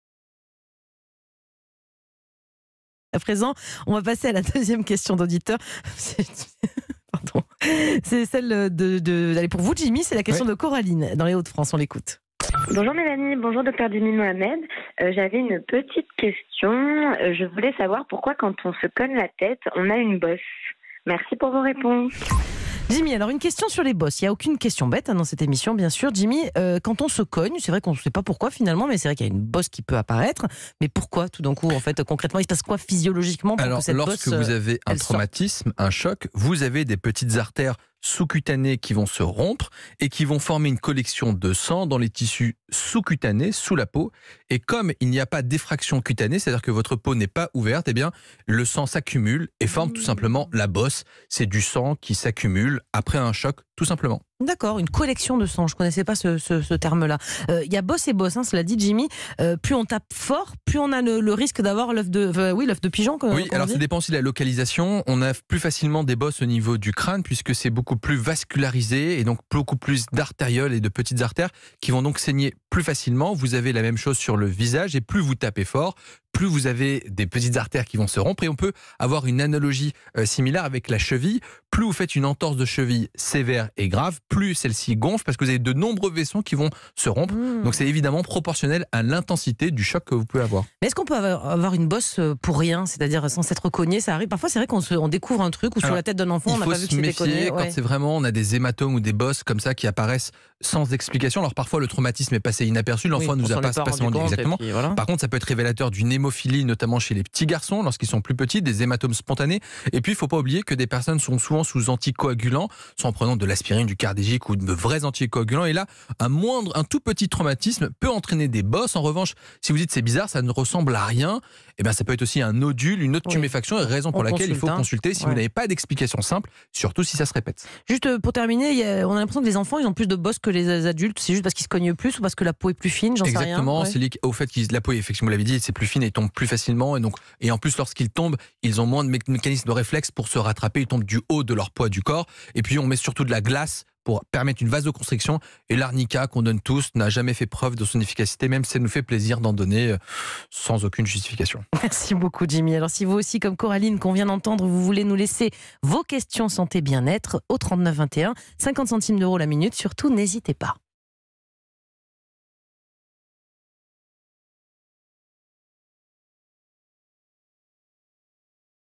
Pourquoi, lorsqu'on se cogne, une bosse apparaît ?
Pour faire face aux multiples interrogations sur le virus ou sur les petits tracas sanitaires du quotidien, le docteur Jimmy Mohamed répond chaque jour aux questions des auditeurs d'Europe 1.